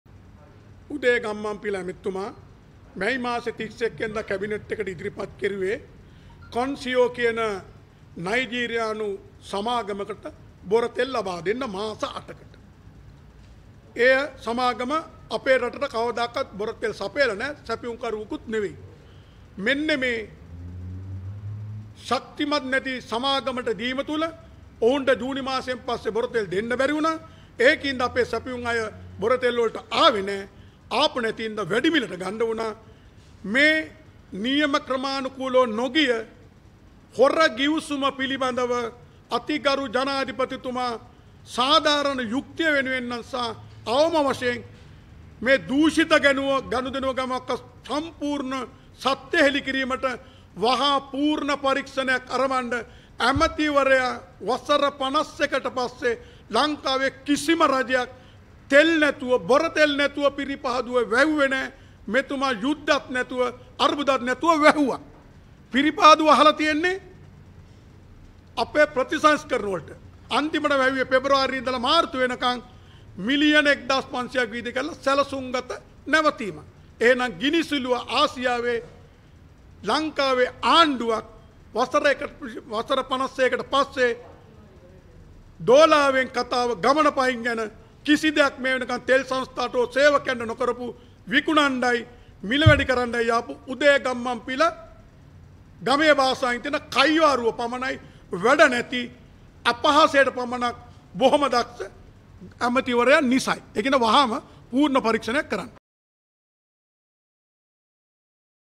The reason for the loss of crude oil
- Ananda Palitha, Secretary, National Employees Union of the Ceylon Petroleum Corporation